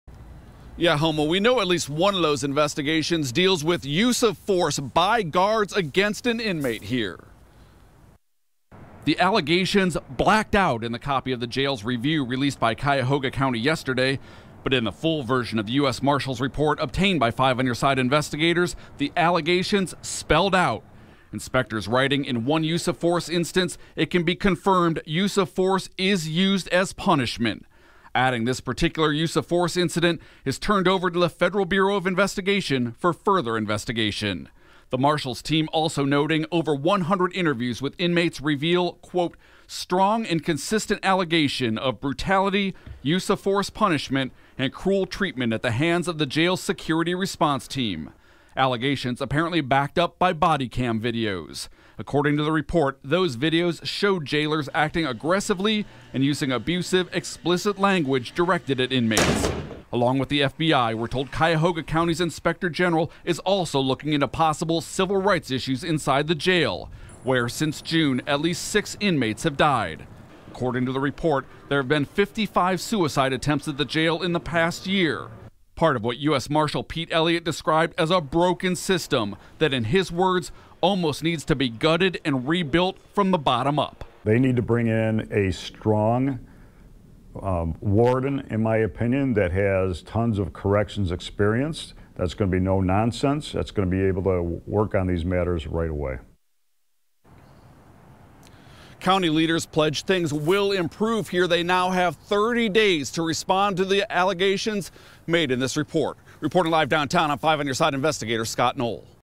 Feds investigating civil rights issue inside Cuyahoga County Jail
The FBI is investigating civil rights issues inside the Cuyahoga County jail according to a damning report detailing serious problems inside the jail.